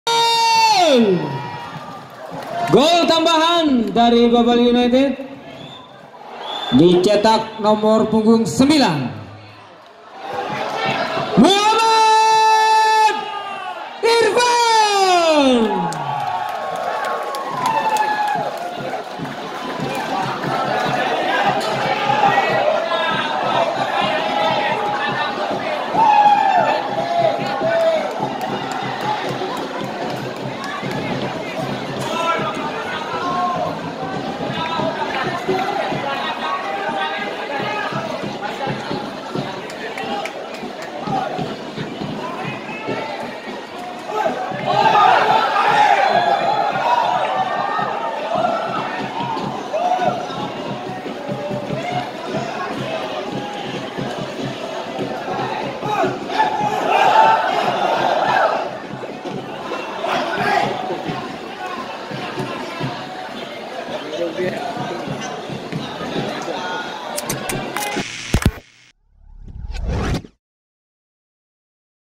Gol Ketiga Babel United Diprotes Pemain dan Pelatih Cilegon United
BANGKAPOS.COM, BANGKA -- Gol Babel United kembali tercipta menit ke 75 ke Gawang Cilegon United. Tandukan M Irvan kembali membuahkan gol dan skor berubah 3-0 keunggulan untuk Babel United.

Namun gol tersebut diprotes oleh pemain dan official Cilegon United. Tampak hakim garis dicerca oleh pemain dan official Cilegon United dengan protes. Namun wasit tetap pada pendiriannya mengesahkan gol.

Kedudukan sementara 3-0 untuk Babel United. Laga berlangusng di Stadion Depati Amir Pangkalpinang Jum'at (23/8/2019). (bangkapos/deddy Marjaya)